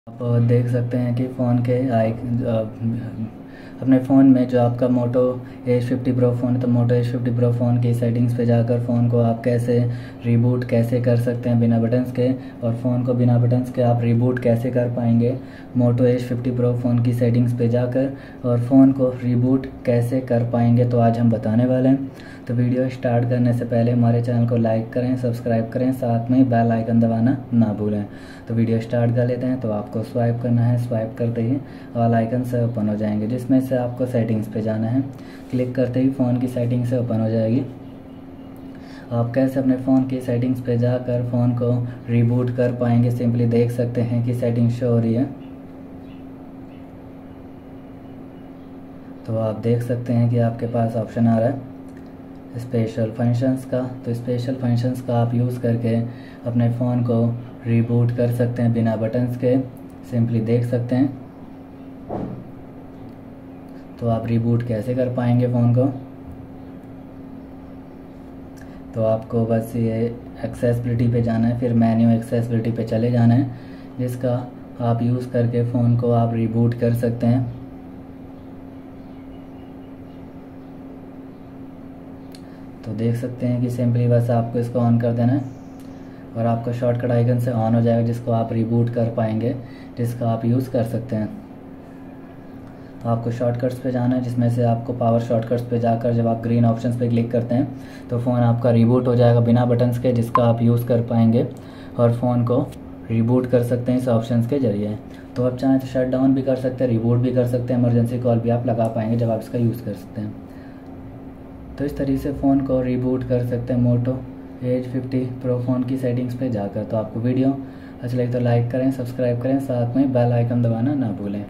Moto edge 50 Pro reboot kaise kare, how to reboot without power button in Moto edge 50 Pro
Best powerful CPU under 10k

60-Inch Tripod with Bag

7 Socket 6A Extension Board

multiplug Extension